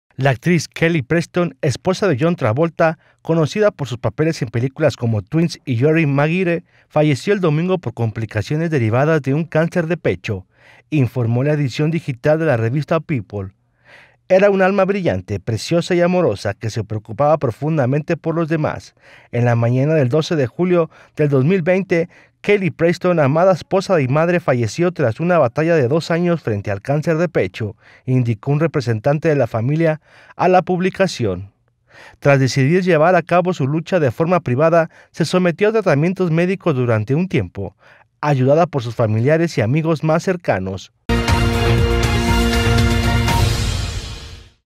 Fallece esposa de Travolta
La actriz de 57 años pierde la lucha contra un cáncer de pecho luego de dos años de tratamiento